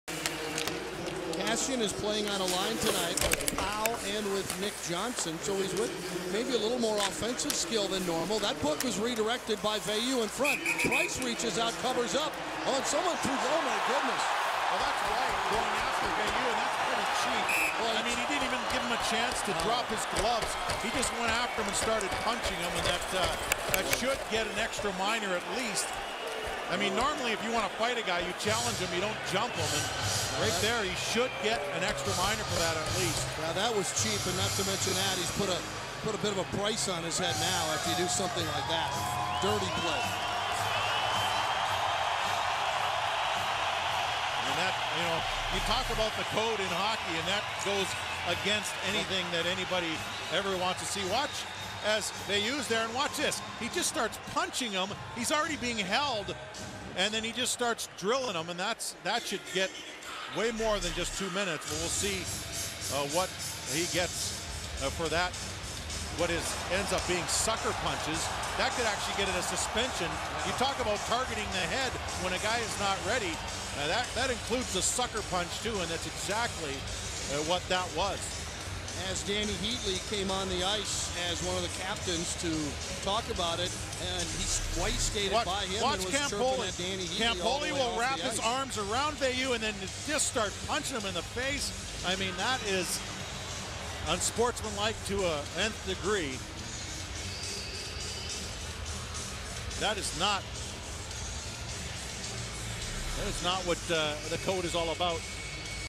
Stephane Veilleux vs Ryan White Round 2 Mar 1, 2012
Stephane Veilleux vs Ryan White Round 2 from the Minnesota Wild at Montreal Canadiens game on Mar 1, 2012.Stephane Veilleux did not receive a fighting major.